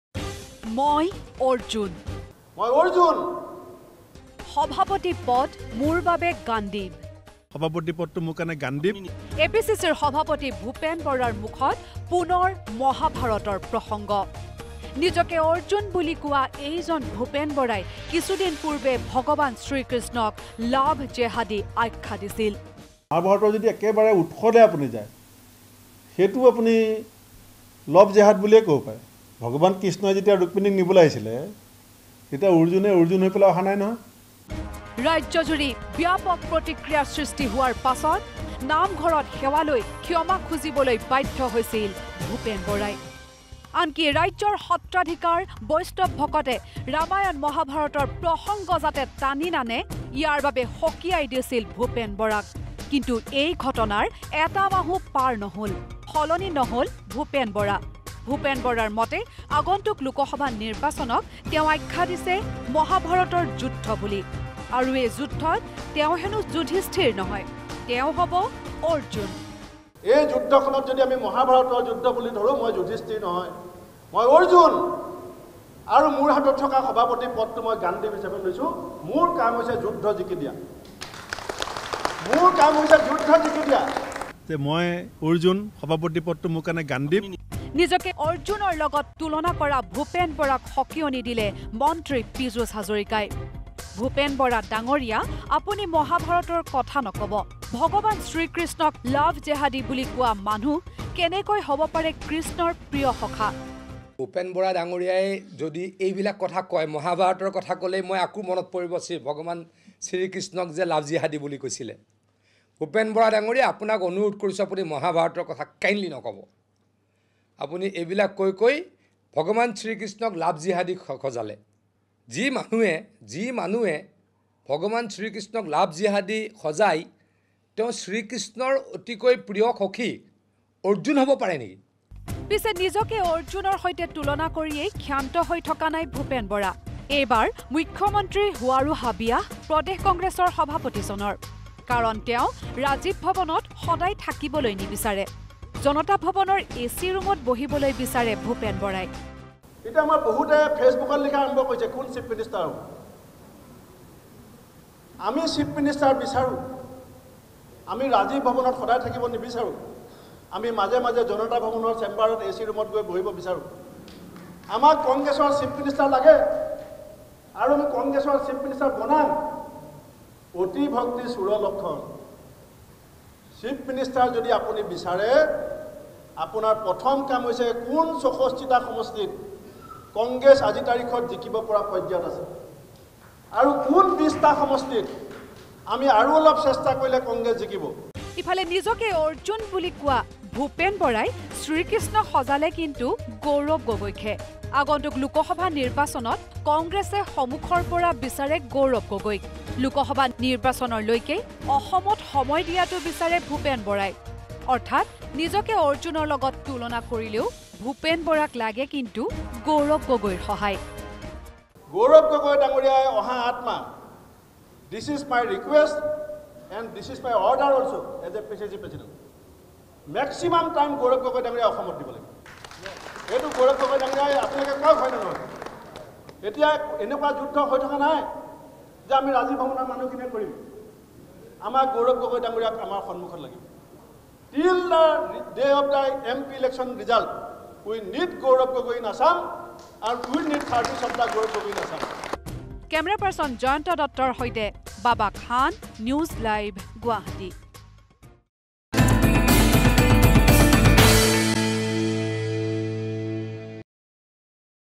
মই অৰ্জুন, শ্ৰীকৃষ্ণক লাভ জেহাদী আখ্যা দিয়া ভূপেন বৰাই এইবাৰ নিজকে অৰ্জুন সজালে
🛑 মই অৰ্জুন। সভাপতি পদ মোৰ বাবে ‘গাণ্ডীৱ’। ভগৱান শ্ৰীকৃষ্ণক লাভ জেহাদী আখ্যা দিয়া ভূপেন বৰাই এইবাৰ নিজকে অৰ্জুন হিচাপে সজাইছে। কেৱল ইমানেই নহয়, গৌৰৱ গগৈক একেসময়তে কৃষ্ণ সজালে কংগ্ৰেছ নেতাগৰাকীয়ে।